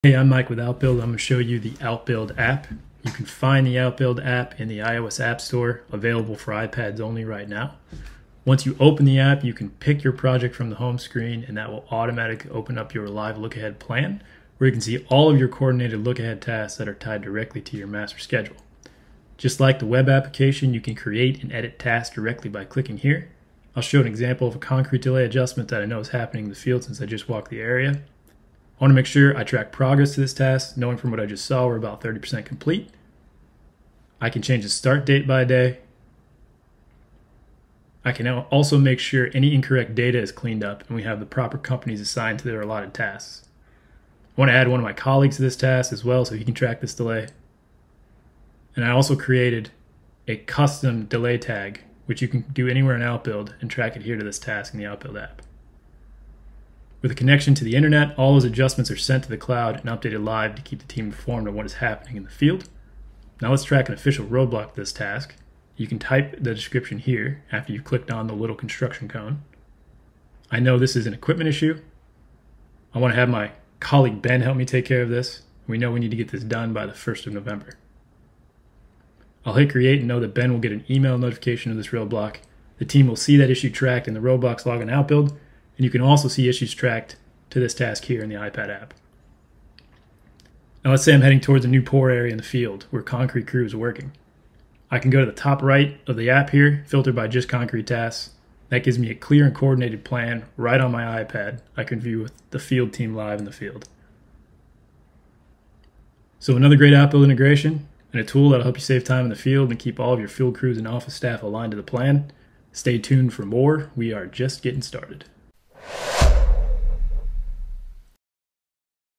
The Simplest Construction Scheduling & Planning App for iPad | Outbuild
Discover the power of the Outbuild iPad app with Mike as he walks you through its functionalities in this video.

Available exclusively on the iOS app store, Outbuild seamlessly integrates with your master schedule, allowing you to manage and track tasks with precision.

Watch as Mike demonstrates live adjustments, such as updating start dates, assigning tasks to the right companies, and collaborating with colleagues. Learn how to tackle delays by adding custom delay tags and incorporating team members into the process.

With real-time cloud updates, the Outbuild app ensures everyone stays informed about on-site developments. Mike showcases how to document roadblocks efficiently, involving team members and generating email notifications.

Explore the roadblocks log on Outbuild to monitor issues collectively. Additionally, discover the app's filtering feature for a focused view of tasks, enhancing coordination between field teams and office staff.

Don't miss out on this introduction to Outbuild's time-saving features – more insights and integrations are on the way.